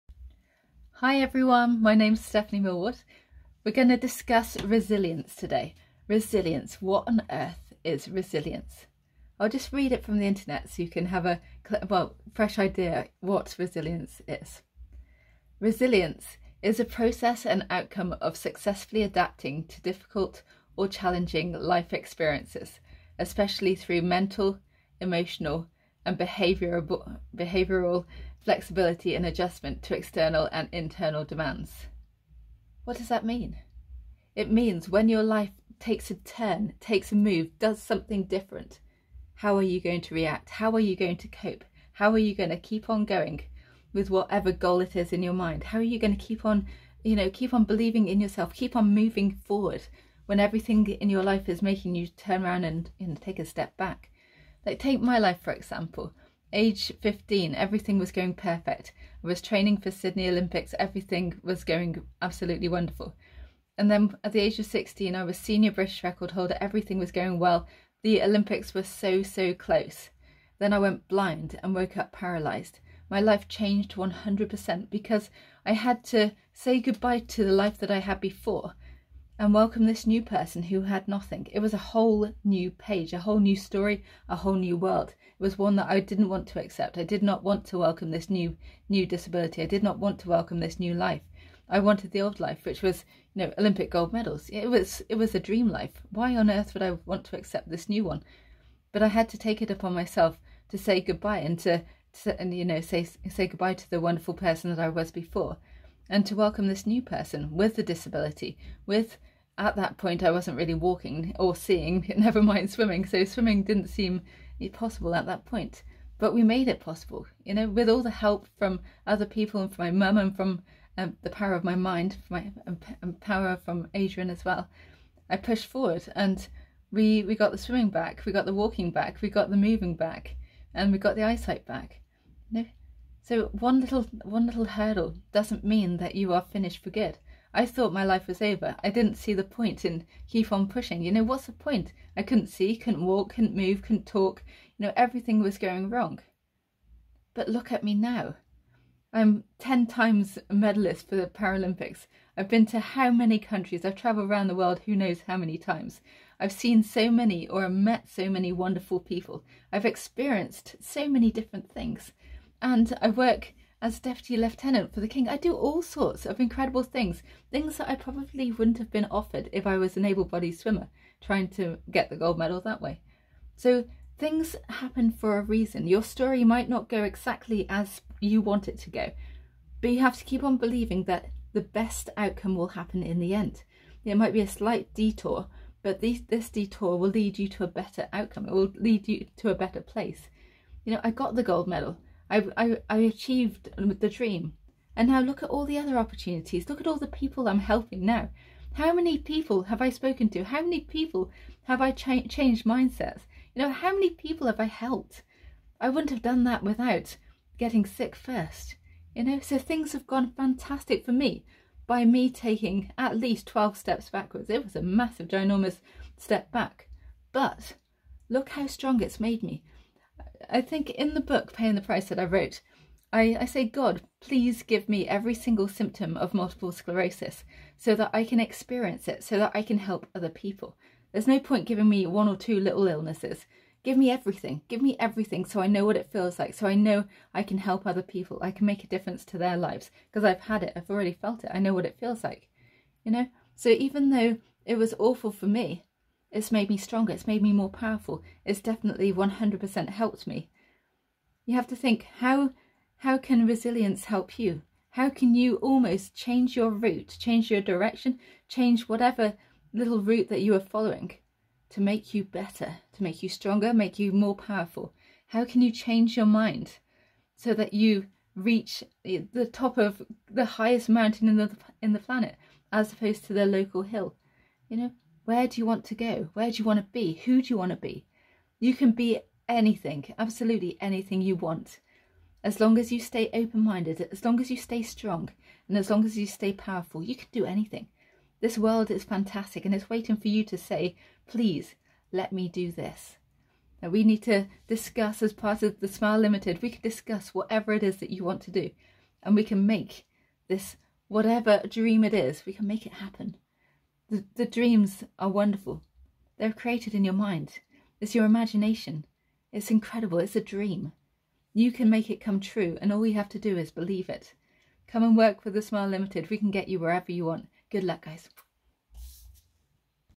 Understanding Resilience!!
What does resilience mean and how does it play a role in The Smile Ltd's Mission?